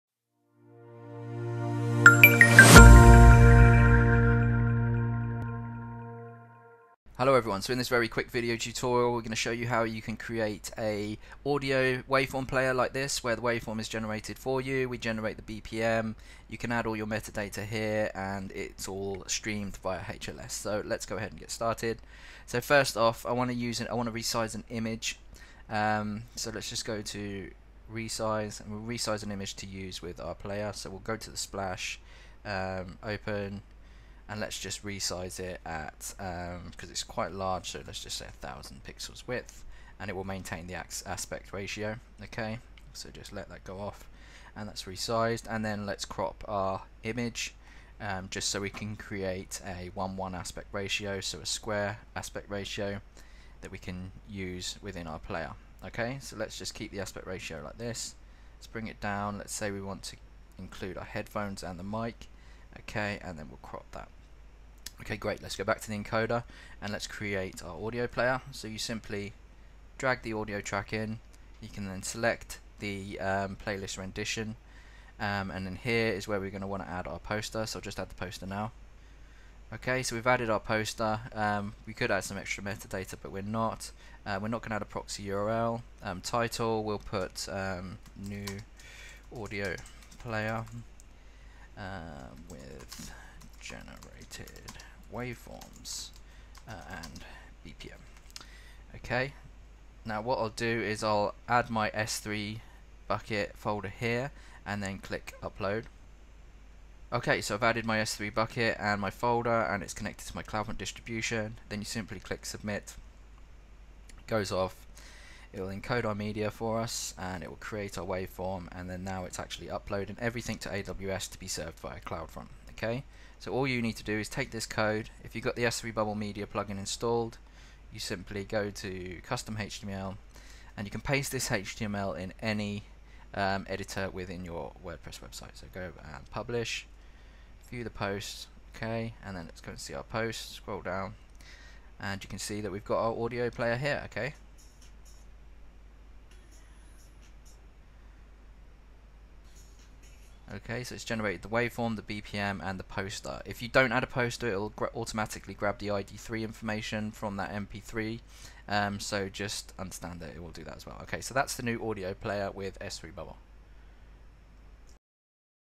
WordPress Audio Waveform Player - S3Bubble
Here we show you our new audio waveform player to create a player similar to Soundcloud but your own custom one for your blog or company.

It streams via HLS and DRM security can be added to prevent your audio from being easily downloaded.